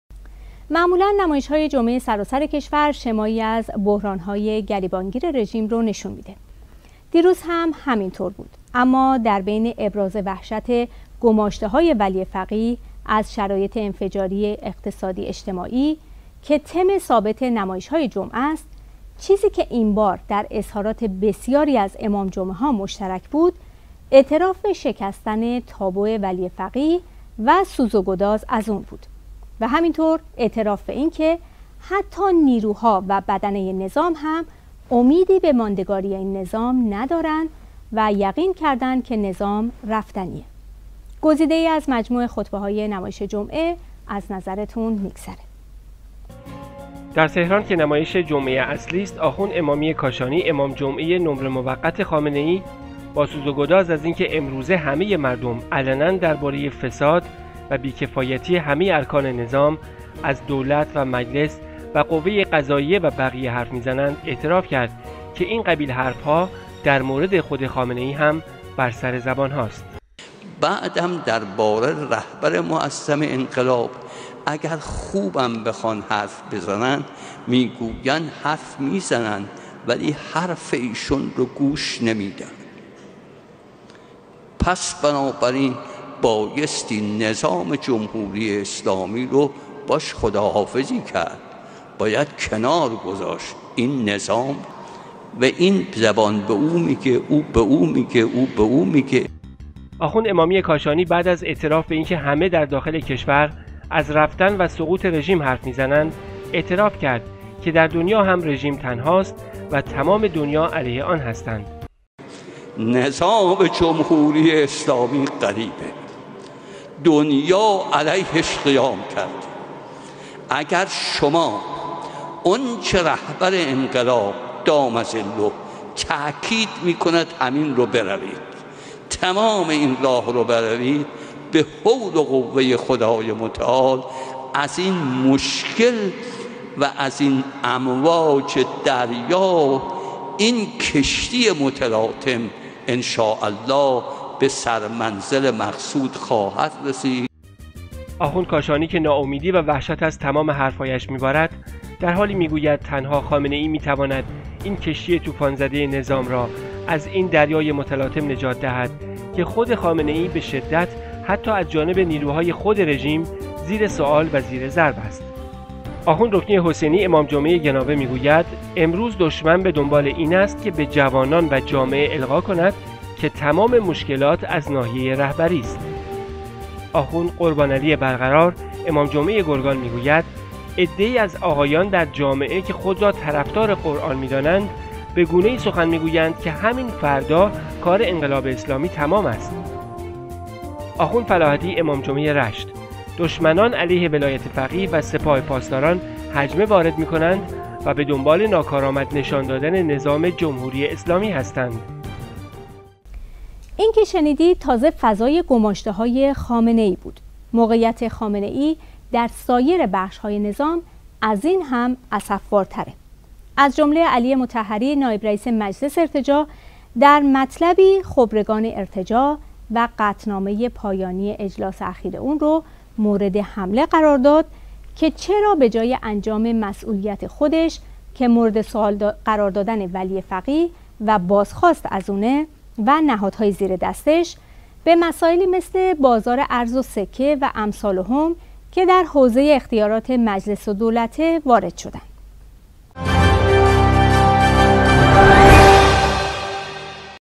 نمازجمعه‌های ایران اعتراف به شکستن تابوی خامنه‌ای
اوجگیری بحران و اعتراف به شکستن تابو خامنه‌ای
معمولاً نمایش‌های جمعه سراسر کشور، شمایی از بحرانهای گریبانگیر رژیم به دست می‌دهد. دیروز هم همین طور بود. اما در میان ابراز وحشت گماشته‌های ولی فقیه از شرایط انفجاری اقتصادی اجتماعی که تم ثابت نمایشهای جمعه است. چیزی که این بار در اظهارات بسیاری از امام جمعه‌ها مشترک بود، اعتراف به شکستن تابو ولی فقیه و سوز و گداز از آن بود و همچنین اعتراف به این که حتی نیروها و بدنة نظام هم امیدی به ماندگاری این نظام ندارند و یقین کرده‌اند که نظام رفتنی است. گزیده‌یی از مجموع خطبه‌های نمایش جمعه از نظرتون می‌گذره:

در تهران که نمایش جمعة اصلی است، آخوند امامی کاشانی امام جمعة نمره موقت خامنه‌ای با سوز و گداز از این که امروزه همه مردم علنا دربارة فساد و بی‌کفایتی همة ارکان نظام از دولت و مجلس و قوة قضاییه و بقیه حرف می‌زنند، اعتراف کرد که این قبیل حرفها در مورد خود خامنه‌ای هم بر سر زبانهاست.
بعد هم درباره رهبر معظم انقلاب اگر خوب هم بخواهند حرف بزنند میگویند حرف میزنند ولی حرف ایشان را گوش نمی دهند پس بنابراین بایستی نظام جمهوری اسلامی را باهاش خداحافظی کرد باید کنار گذاشت این نظام و این زبان به آن میگه او به او میگه او به او میگه».

آخوند امامی کاشانی بعد از اعتراف به این که همه در داخل کشور از رفتن و سقوط رژیم حرف می‌زنند اعتراف کرد که در دنیا هم رژیم تنهاست و تمام دنیا علیه آن هستند:
جمهوری اسلامی غریب است دنیا علیه اش قیام کرده اگر شما آنچه رهبر انقلاب دام ضله تاکید میکند همین را بروید تمام این راه را بروید به حول قوه خدای متعال از این مشکل و از این امواج دریا این کشتی متلاطم انشاالله به سر منزل مقصود خواهد رسید».

آخوند کاشانی که ناامیدی و وحشت از تمام حرفهایش می‌بارد، در حالی می‌گوید تنها خامنه‌ای می‌تواند این کشتی طوفان زدة نظام می‌تواند از این دریای متلاطم نجات دهد که خود خامنه‌ای به شدت حتی از جانب نیروهای خود رژیم زیر سؤال و زیر ضرب است.
آخوند رکنی حسینی  امام جمعه گناوه می‌گوید:‌ امروز دشمن به دنبال این است که به جوانان و جامعه القا کند که تمام مشکلات از ناحیه رهبری است
آخوند قربانعلی برقرار، امام جمعه گرگان می‌گوید: عده ای از آقایان در جامعه که خود را طرفدار قرآن می دانند به گونه‌ای سخن می گویند که همین فردا کار انقلاب اسلامی تمام است
آخوند فلاحتی امام جمعه رشت: دشمنان علیه ولایت فقیه و سپاه پاسداران هجمه وارد می کنند و به دنبال ناکارآمد نشان دادن نظام جمهوری اسلامی هستند.

این که شنیدید، تازه فضای گماشته‌های خامنه‌ای بود؛ موقعیت خامنه‌ای در سایر بخشهای نظام، از این هم اسفبارتر است. از جمله علی مطهری نایب رئیس مجلس ارتجاع، در مطلبی خبرگان ارتجاع و قطعنامة‌ پایانی اجلاس اخیر آن را مورد حمله قرار داد که چرا به جای انجام مسئولیت خود که مورد سؤال قرار دادن ولی فقیه و بازخواست از او و نهادهای زیر دستش می‌باشد، به مسائلی مانند بازار ارز و سکه و امثالهم که در حوزة اختیارات مجلس و دولت است، وارد شده‌اند؟!